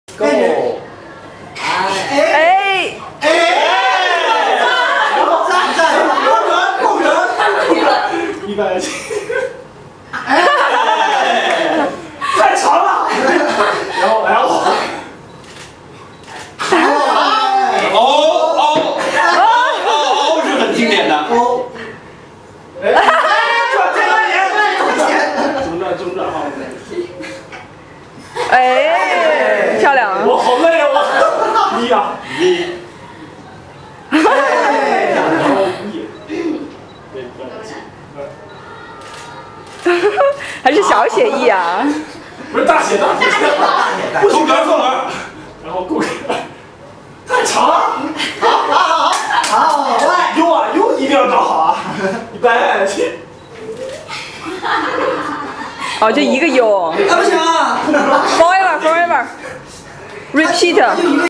Games on the wedding
It is a tradition that people will ask bride and bridegroom to do some games in Chinese wedding